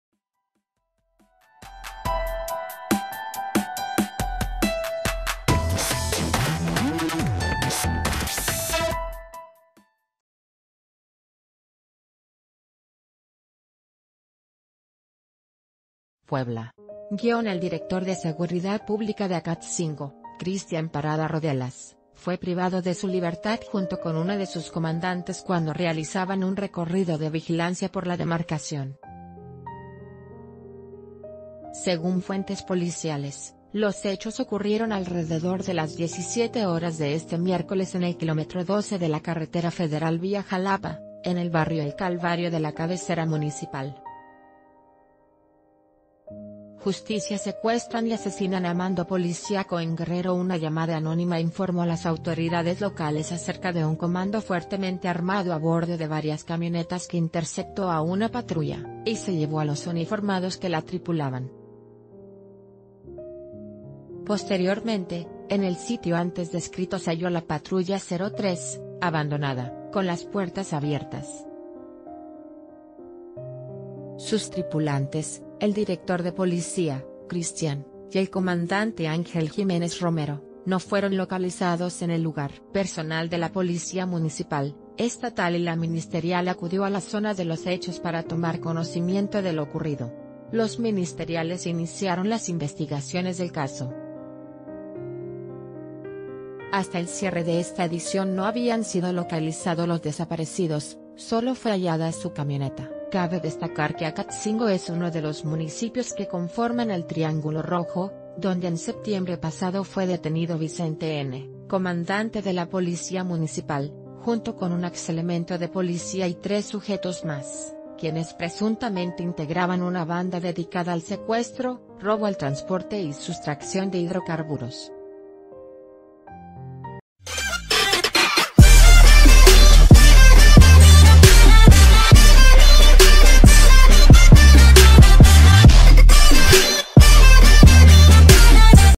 Secuestran a director de Seguridad y un comandante de Acatzingo, Puebla
Puebla.- El director de Seguridad Pública de Acatzingo, Christian Parada Rodelas, fue privado de su libertad junto con uno de sus comandantes cuando realizaban un recorrido de vigilancia por la demarcación. Según fuentes policiales, los hechos ocurrieron alrededor de las 17:00 horas de este miércole...